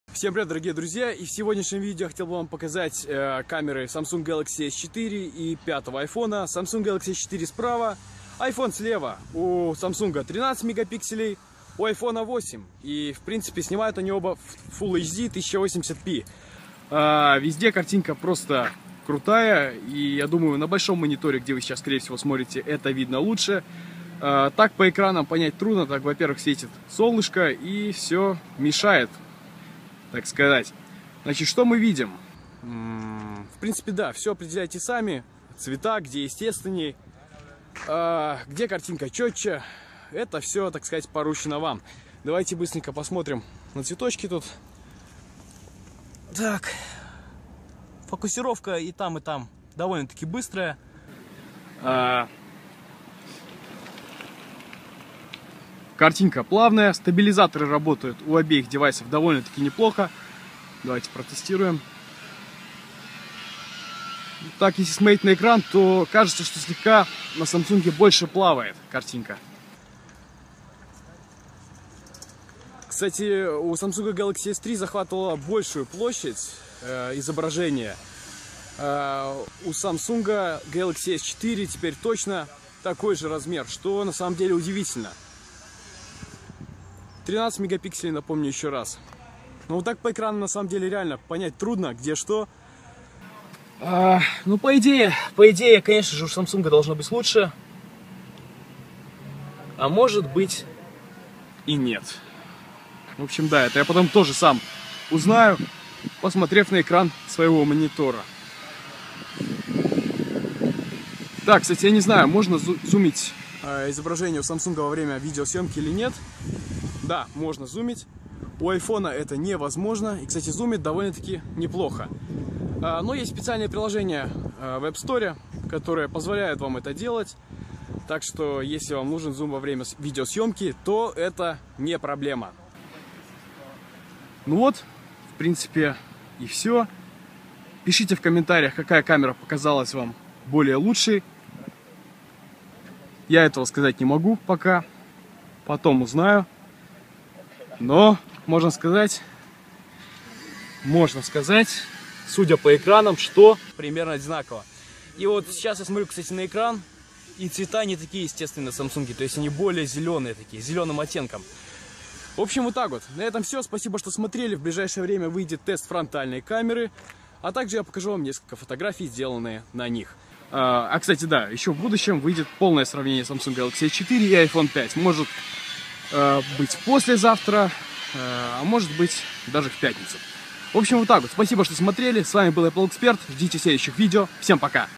Сравнение съёмки видео с задней камеры Samsung Galaxy S4 и iPhone 5
GameCenter: APPLEEXPERT